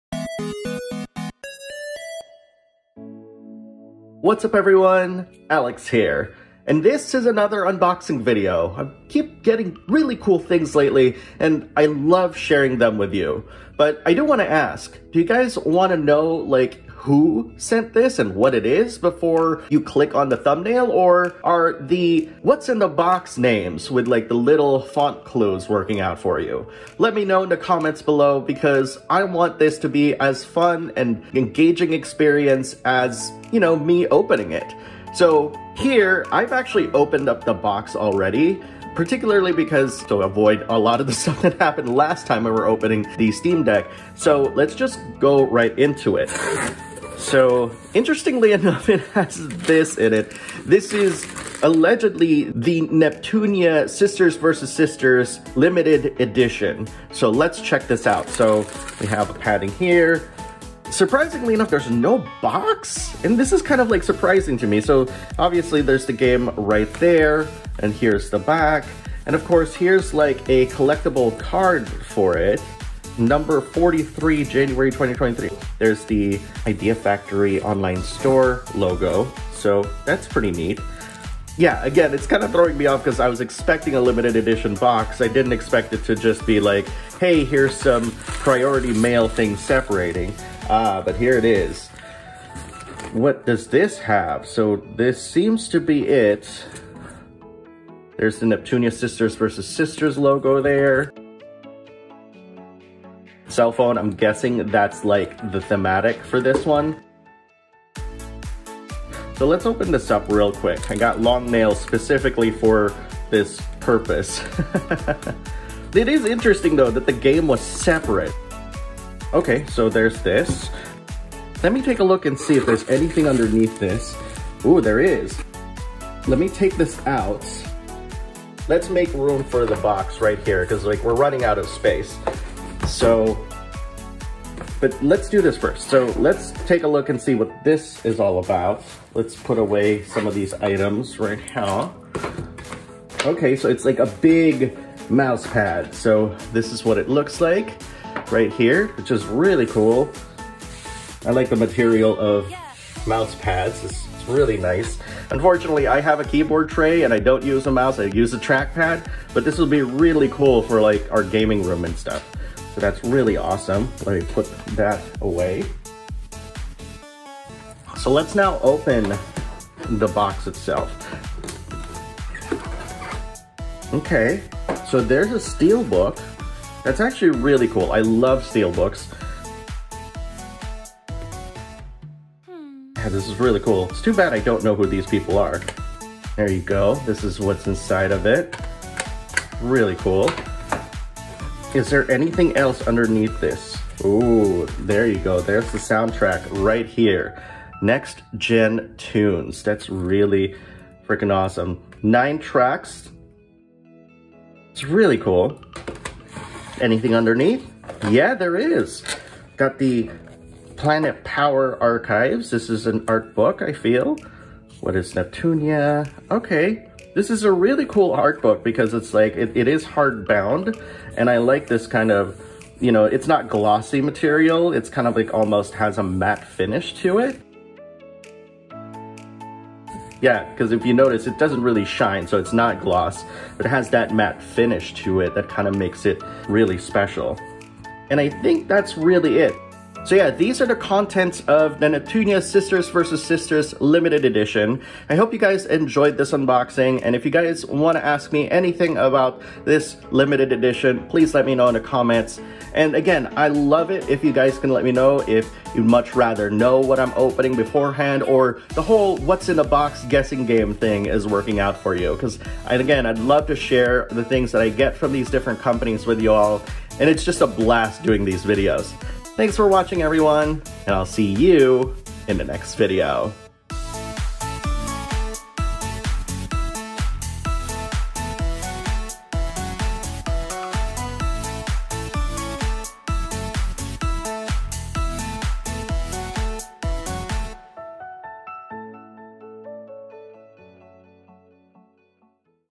A package from Idea Factory? LET'S OPEN IT TOGETHER! | Backlog Battle
And if you're looking at that logo thinking, "Hey, that's just a pink Mass Effect logo!" well, I tried my best to emulate the original - even downloading the danged font. Gimme slack here, mmk?